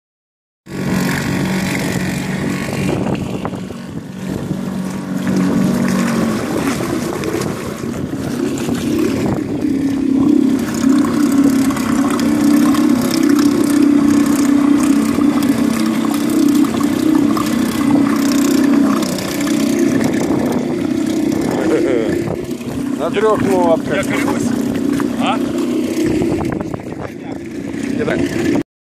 Что делать когда мотор только на 1 лодке
Перемещаемся на буксире, на речке Южный Буг. Мотор Yamaha 5 л.с.